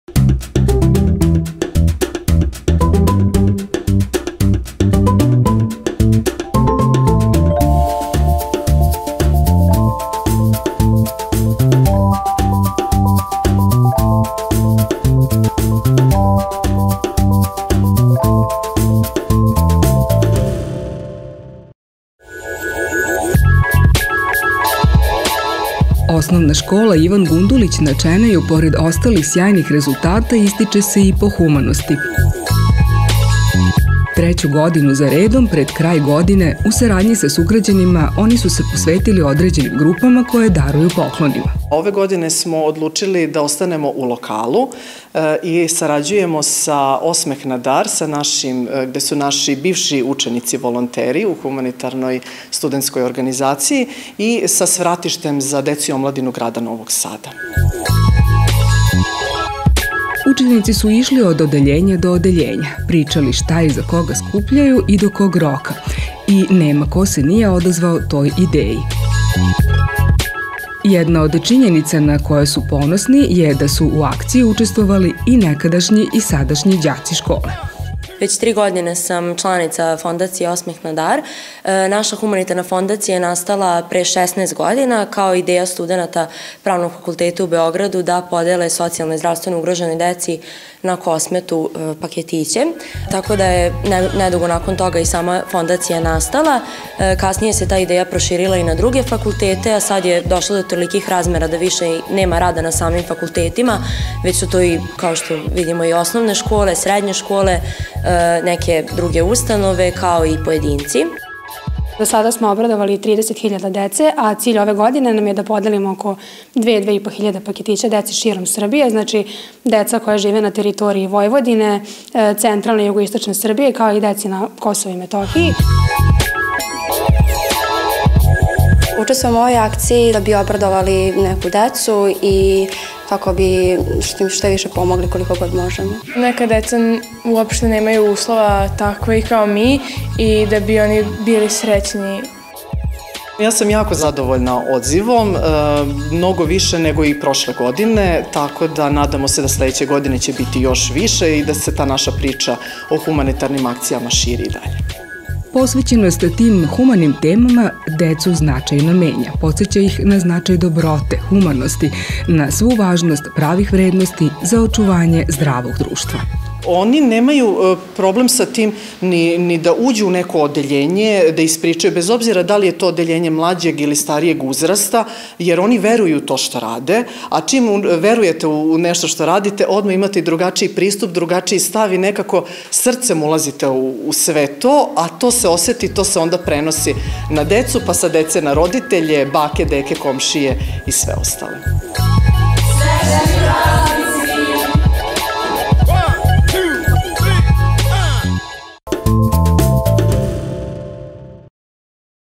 Komšijske priče: Humanitarne akcije u školi "Ivan Gundulić" Čenej
Ovo je već treća godina kako u Osnovnoj školi "ivan Gundulić" na Čeneju organizuju humanitarne akcije, kako bi pred kraj godine obradovali svu decu koja za to nemaju priliku koliko imaju oni. Još jedan lep momenat njihove inicijative je da u akciji učestvuju i nekadašnji učenici škole.